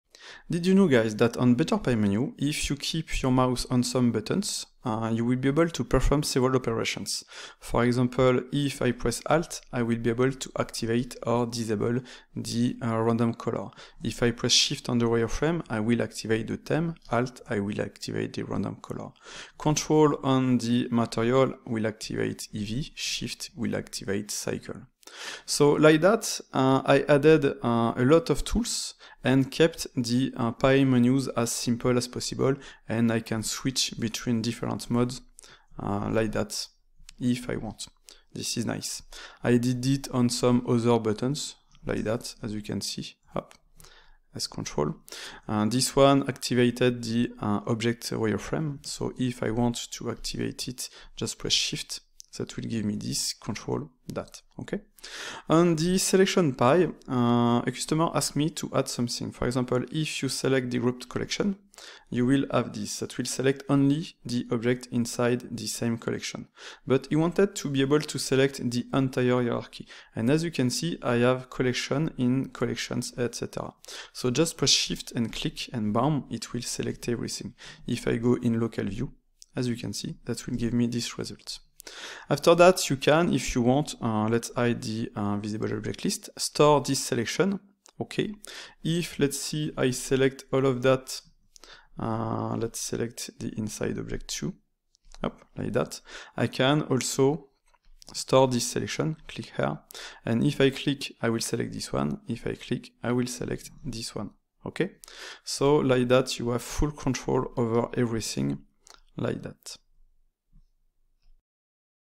Better Pie Menus - Wizard mode in Blender
On better pie menus add-on, you can perform several operations with a single button.

You can keep it simple or work like a wizard  🧙🪄